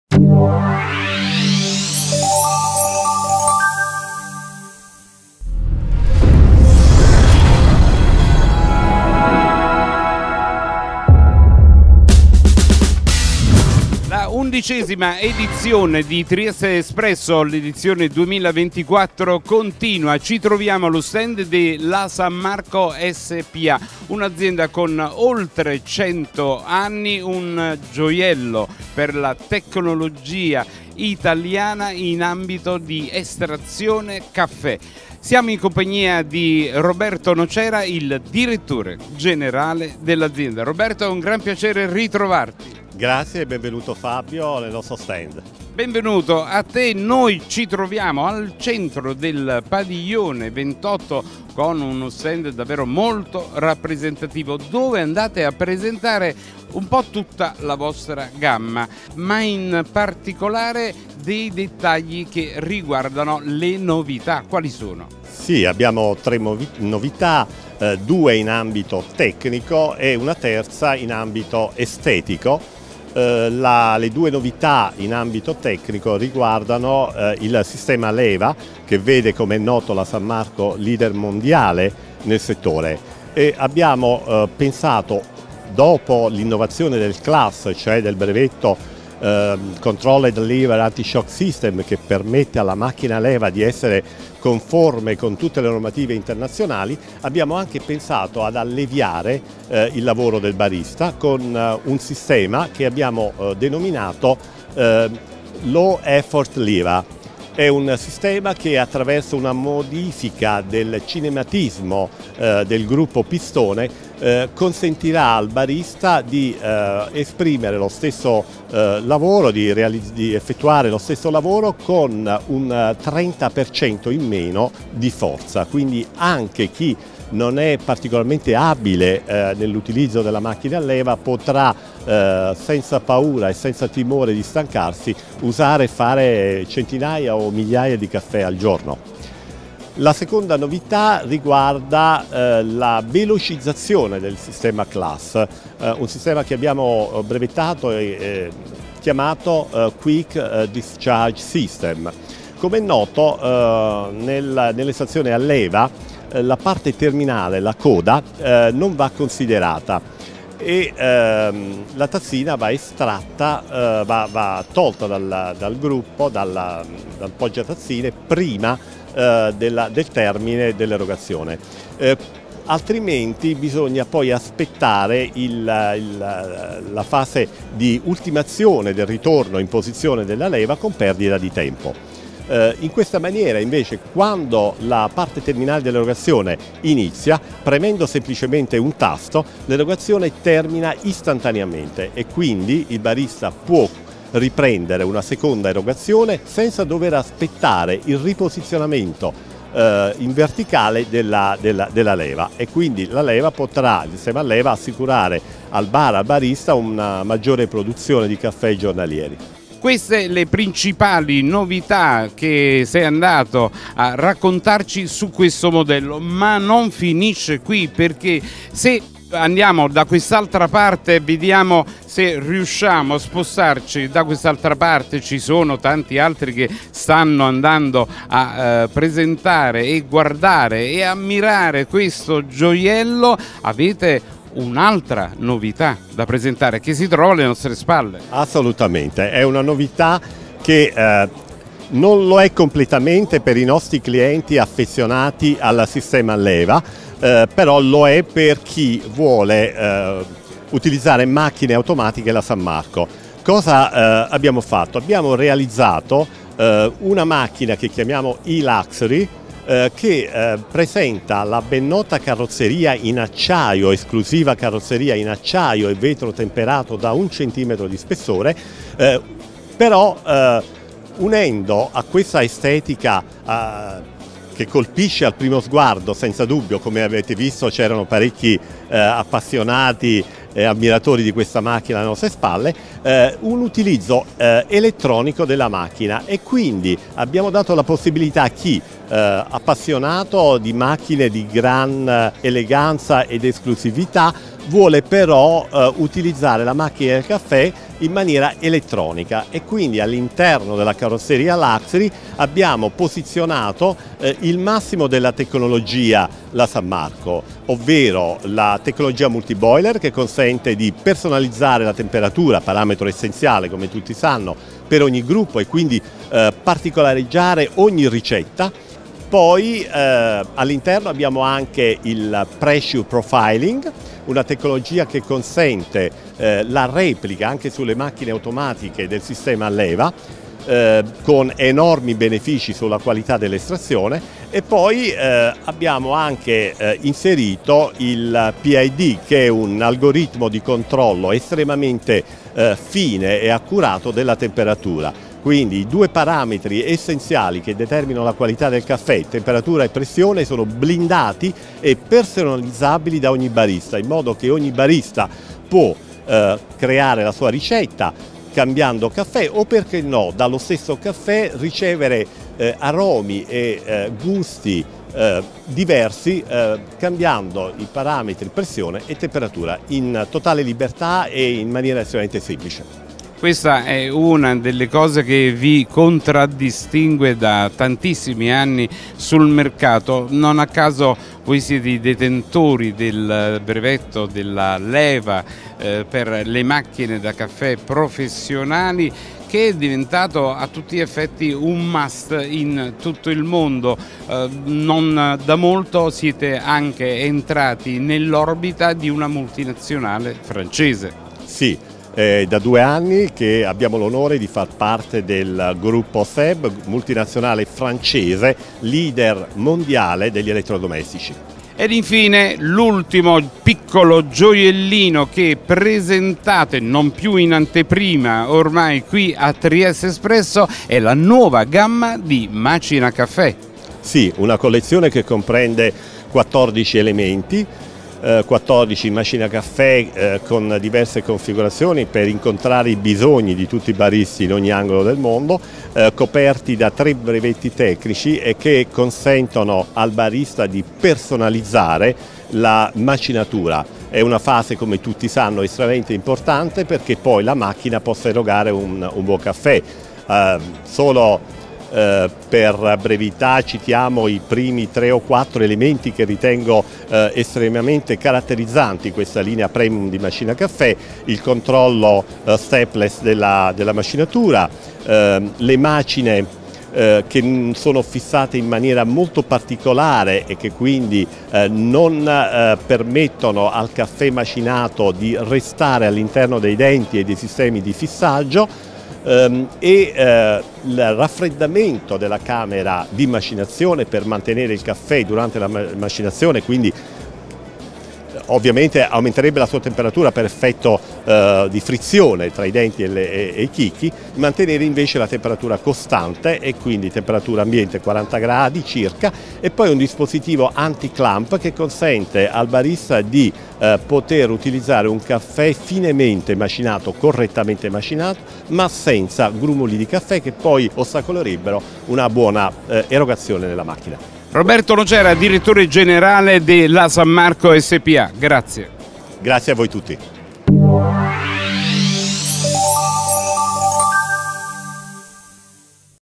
TRIESTESPRESSO 2024 - Intervista con Roberto Nocera, Direttore Generale di La San Marco SpA
Torniamo a Triestespresso Expo 2024, dove Fabio Russo ha intervistato per HorecaTv Roberto Nocera, Direttore Generale di La San Marco SpA che ai nostri microfoni ha presentato tutte le novità che l'azienda, eccellenza made in Italy e leader nel mondo nella produzione di macchine per il caffè, ha portato in fiera.
Due sono le novità "tecniche", la prima riguarda il sistema Leva: La San Marco ha presentato il fiera la tecnologia Low Effort Leva (LEL), che consente al barista di effettuare lo stesso lavoro con il 30% in meno di forza, permettendo di mantenere un ritmo sostenuto nel lavoro con la macchina anche se non si ha particolare esperienza nel maneggiarla.
Altra novità è il Quick Discharge System (QDS), un'innovazione che perfeziona la fase finale di estrazione del caffè, riducendo al minimo la coda finale e consentendo al barista di effettere una seconda erogazione senza dover aspettare il riposizionamento in verticale della leva. Questo sistema può consentire in tal modo la realizzazione di un maggior numero di caffè.

La San Marco ha presentato inoltre a Trieste un nuovo modello di macchina da caffè, E.LUXURY, sintesi perfetta tra tradizione e innovazione con un'estetica unica e raffinata caratterizzata da una carrozzeria in acciaio e vetro temperato abbinata all'innovazione del funzionamento elettronico.
La macchina è dotata di tecnologia multiboiler con cui personalizzare la temperatura per ogni gruppo. Il pressure profiling consente inoltre la replica anche sulle macchine automatiche del sistema a leva, migliorando la qualità dell'estrazione, mentre l'algoritmo PID permette il controllo della temperatura.
In questa macchina i due parametri essenziali che determinano la qualità del caffè, temperatura e pressione, sono personalizzabili da ogni barista in modo molto semplice.
Presente inoltre la collezione di macinacaffè La San Marco - coperti da tre brevetti tecnici -  composta da 14 modelli che vogliono rispondere ai bisogni di tutti i baristi del mondo e che consentono di personalizzare la macinatura.